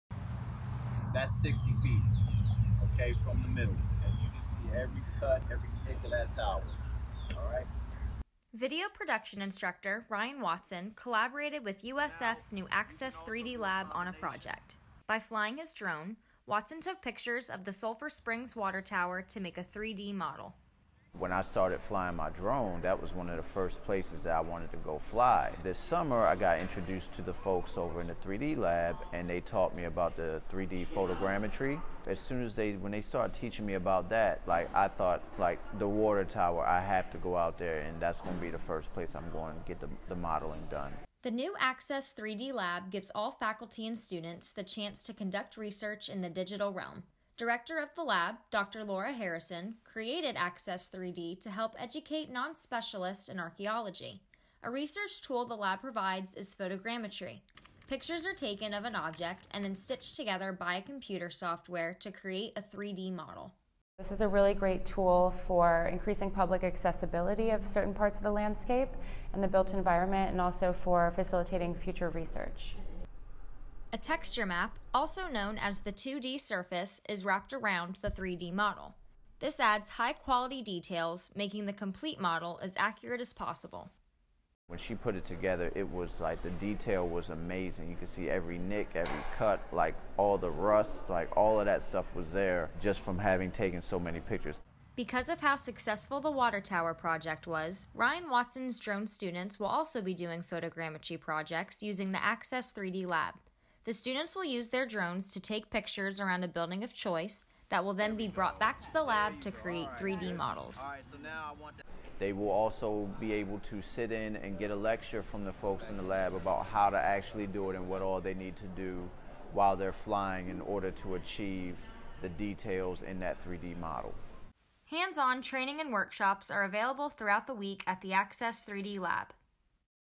Tampa Water Tower Photogrammetry Project
By Natalie Eiland

The University of South Florida’s new Access 3D Lab helps conduct research in the digital realm and is open to all faculty and students of USF.

People can use 3D scanners in the lab. The lab also works on projects like digital sculpting, photogrammetry, creation of virtual museums and forensic analysis.

Dr. Laura Harrison, director of the Access 3D Lab, is a research assistant professor at USF. She was inspired to create the 3D Access Lab due to her interest in cultural heritage preservation. Originally trained as a Bronze Age archaeologist, Harrison became more interested in 3D virtualization while doing research at a threatened site in Turkey. She aims to educate non-specialists and children in archaeological heritage.

Students and faculty can use the 3D Access Lab’s equipment for their own research. The lab offers training and workshops on equipment. Individual or group lessons are available along with courses that range from three to five days. In order to complete the course, it takes about 10 to 20 hours of hands-on training. The lab is currently working on several project collaborations.

USF’s video production instructor, Ryan Watson, is experienced in videography and drone flying. He worked with the 3D Access Lab on a photogrammetry project. Watson flew his drone around the Sulphur Springs Water Tower located in Tampa. He took hundreds of pictures from different heights and angles. These pictures were then taken back to the 3D Access Lab and were stitched together using the photogrammetry software at the lab’s computer workstations.

To achieve the most detailed and high-quality finished product, the computer software Agisoft PhotoScan generated a texture map. The map contains the 2-dimensional layout of the landscape around the water tower, incorporating color information used to make the 3-dimensional model. This texture map applied that information to the water tower model which contributed to creating the finished model with the highest quality of detail. The lab was able to get the most accurate data from the finished product.

Watson also has a drone course at USF that teaches students how to fly drones to take pictures and videos. The class will be working with the 3D Access Lab by conducting the same kind of project as the Tampa water tower but will fly around a building of their own choice. Before taking the pictures with their drones, the students will sit through a lecture given by some of the instructors of the 3D Access Lab on how to take the best pictures to get the most detailed finished product. The pictures that the students take will be brought back to the 3D Access Lab where the students themselves will learn how to stitch the pictures together using the photogrammetry software.

This story was created by USF student Natalie Eiland for Jeanette Abrahamsen's Advanced Reporting class in 2018.